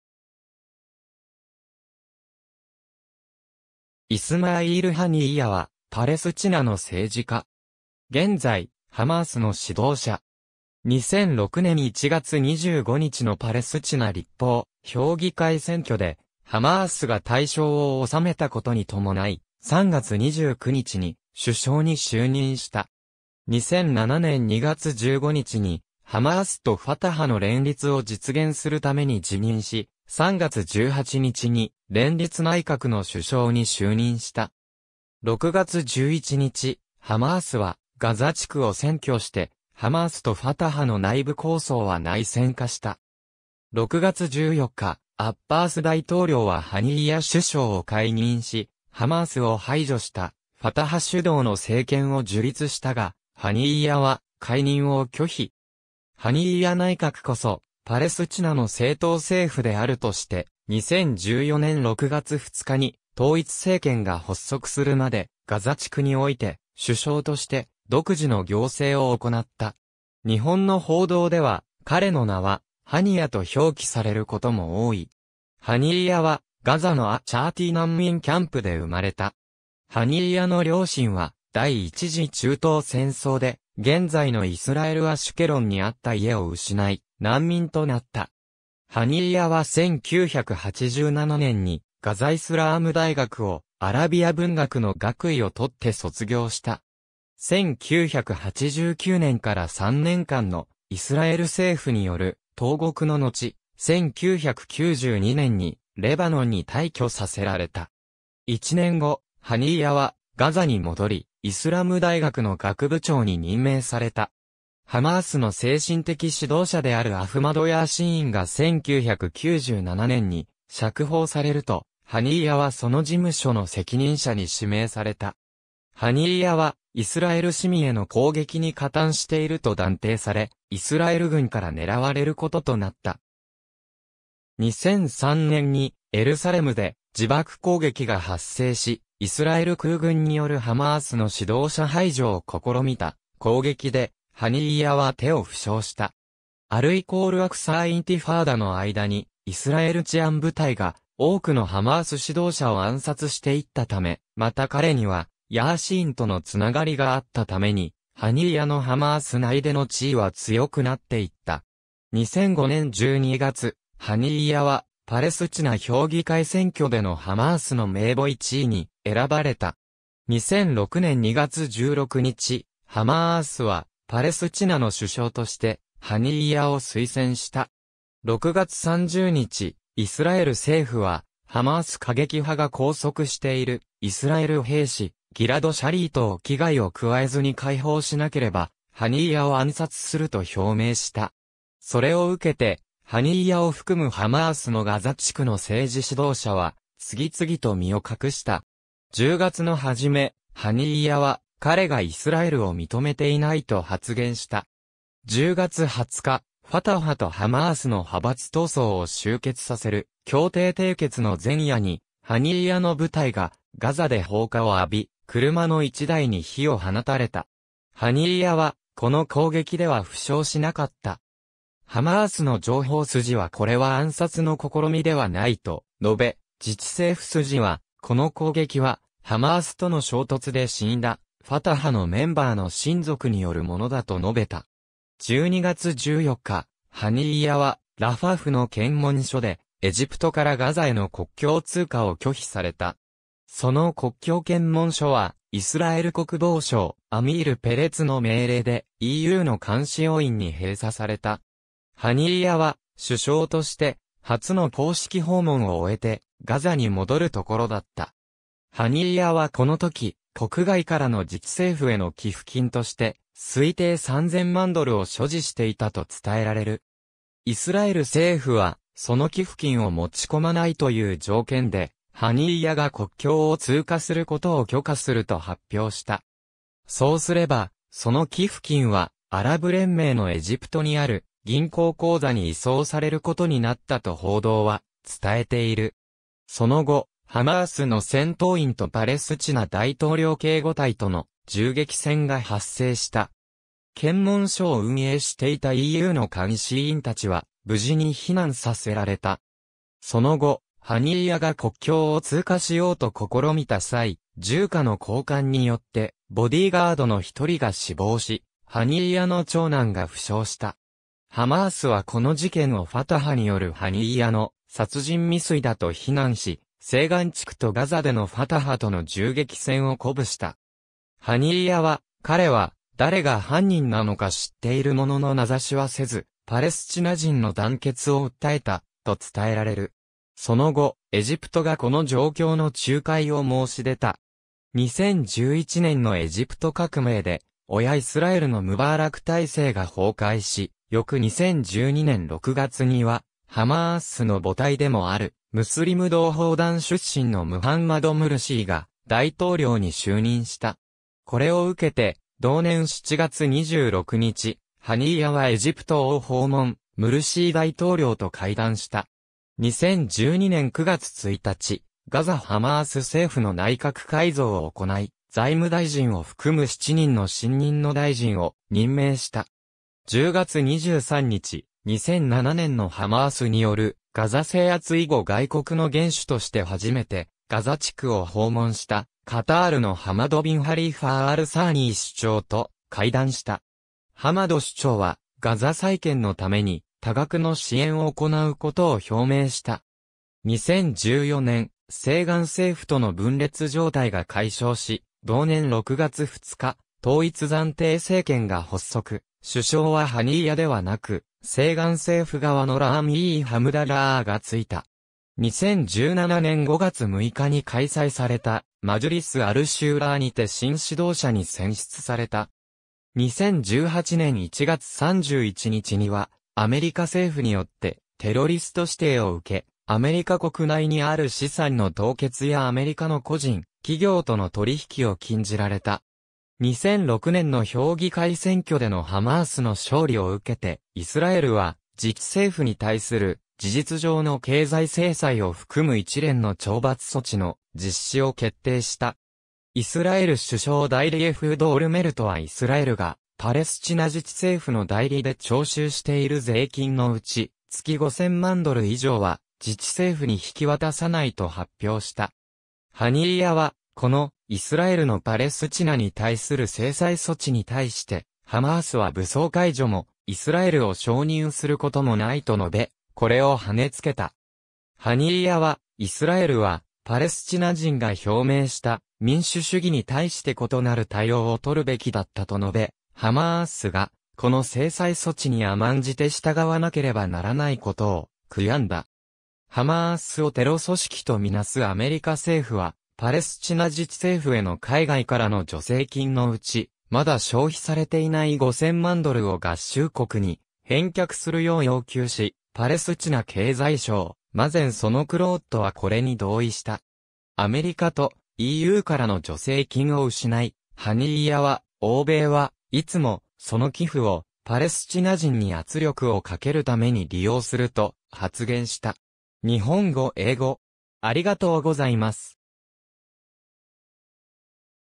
イスマーイール・ハニーヤ
イスマーイール・ハニーヤ（アラビア語: إسماعيل هنية‎、Ismaʻīl Haniyya、1963年1月29日 - ）は、パレスチナの政治家。
現在、ハマースの指導者。
2006年1月25日のパレスチナ立法評議会選挙でハマースが大勝を収めたことにともない、3月29日に首相に就任した。
2007年2月15日にハマースとファタハの連立を実現するために辞任し、3月18日に連立内閣の首相に就任した。
6月11日、ハマースはガザ地区を占拠して、ハマースとファタハの内部抗争は内戦化した。
6月14日、アッバース大統領はハニーヤ首相を解任し、ハマースを排除したファタハ主導の政権を樹立したが、ハニーヤは解任を拒否。
ハニーヤ内閣こそ、パレスチナの正当政府であるとして、2014年6月2日に統一政権が発足するまでガザ地区において首相として独自の行政を行った。
日本の報道では彼の名はハニヤと表記されることも多い（NHKのみハニーヤと表記）。
ハニーヤはガザのアッ・シャーティー難民キャンプで生まれた。
ハニーヤの両親は第一次中東戦争で現在のイスラエル・アシュケロンにあった家を失い難民となった。
ハニーヤは1987年にガザ・イスラーム大学をアラビア文学の学位をとって卒業した。
1989年から3年間のイスラエル政府による投獄ののち、1992年にレバノンに退去させられた。
1年後、ハニーヤはガザに戻り、イスラム大学の学部長に任命された。
ハマースの精神的指導者であるアフマド・ヤースィーンが1997年に釈放されると、ハニーヤはその事務所の責任者に指名された。
ハニーヤはイスラエル市民への攻撃に加担していると断定され、イスラエル軍から狙われることとなった。
2003年にエルサレムで自爆攻撃が発生し、イスラエル空軍によるハマースの指導者排除を試みた攻撃でハニーヤは手を負傷した。
アル＝アクサー・インティファーダの間にイスラエル治安部隊が多くのハマース指導者を暗殺していったため、また彼にはヤースィーンとの繋がりがあったために、ハニーヤのハマース内での地位は強くなっていった。
2005年12月、ハニーヤはパレスチナ評議会選挙でのハマースの名簿1位に選ばれた。
2006年2月16日、ハマースはパレスチナの首相としてハニーヤを推薦した。
6月30日、イスラエル政府はハマース過激派が拘束しているイスラエル兵士ギラド・シャリートを危害を加えずに解放しなければハニーヤを暗殺すると表明した。
それをうけて、ハニーヤを含むハマースのガザ地区の政治指導者は次々と身を隠した。
10月の初め、ハニーヤは彼がイスラエルを認めていないと発言した。
10月20日、ファタハとハマースの派閥闘争を終結させる協定締結の前夜に、ハニーヤの部隊がガザで放火を浴び、車の1台に火を放たれた。
ハニーヤはこの攻撃では負傷しなかった。
ハマースの情報筋はこれは暗殺の試みではないと述べ、自治政府筋はこの攻撃はハマースとの衝突で死んだファタハのメンバーの親族によるものだと述べた。
12月14日、ハニーヤはラファフの検問所でエジプトからガザへの国境通過を拒否された。
その国境検問所はイスラエル国防相アミール・ペレツの命令でEUの監視要員に閉鎖された。
ハニーヤは首相として初の公式訪問を終えてガザに戻るところだった。
ハニーヤはこのとき、国外からの自治政府への寄付金として推定3000万ドルを所持していたと伝えられる。
イスラエル政府はその寄付金を持ち込まないという条件で、ハニーヤが国境を通過することを許可すると発表した。
そうすれば、その寄付金はアラブ連盟のエジプトにある銀行口座に移送されることになったと報道は伝えている。
その後、ハマースの戦闘員とパレスチナ大統領警護隊との銃撃戦が発生した。
検問所を運営していたEUの監視員たちは無事に避難させられた。
その後、ハニーヤが国境を通過しようと試みた際、銃火の交換によってボディーガードの1人が死亡し、ハニーヤの長男が負傷した。
ハマースはこの事件をファタハによるハニーヤの殺人未遂だと非難し、西岸地区とガザでのファタハとの銃撃戦を鼓舞した。
ハニーヤは、彼は誰が犯人なのか知っているものの名指しはせず、パレスチナ人の団結を訴えた、と伝えられる。
その後、エジプトがこの状況の仲介を申し出た。
2011年のエジプト革命で親イスラエルのムバーラク体制が崩壊し、翌2012年6月にはハマースの母体でもあるムスリム同胞団出身...